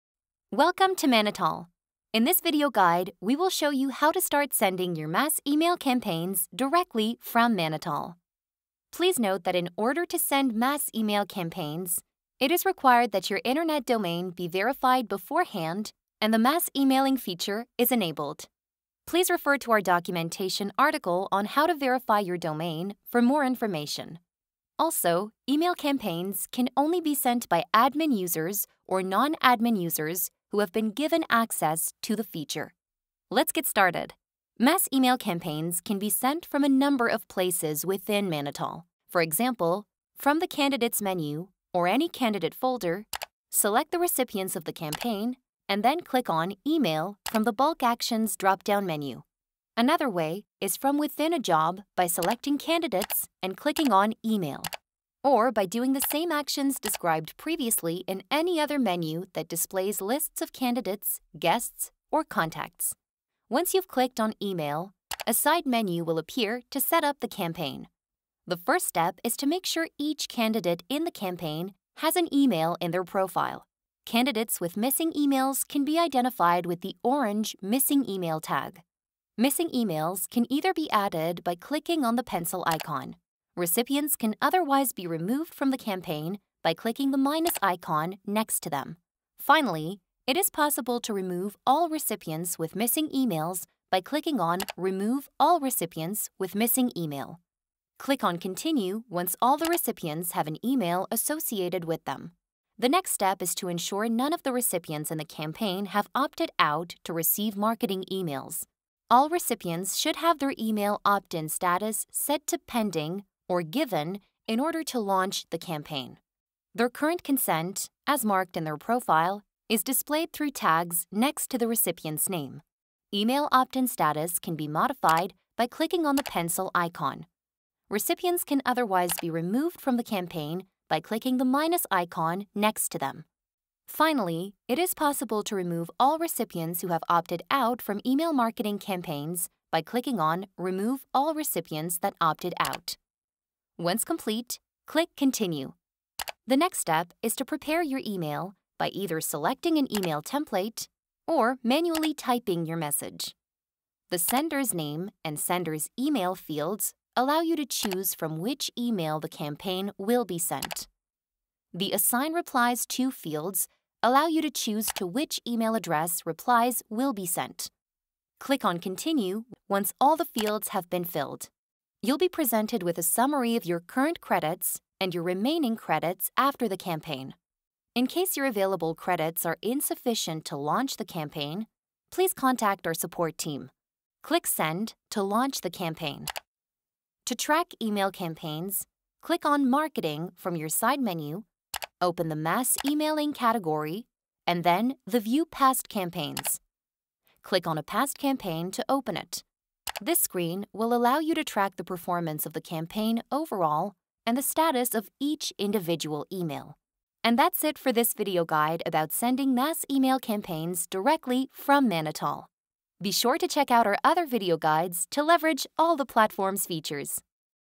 Sending Mass Emailing Campaigns
Send mass email campaigns to hundreds of recipients directly from your Manatal account.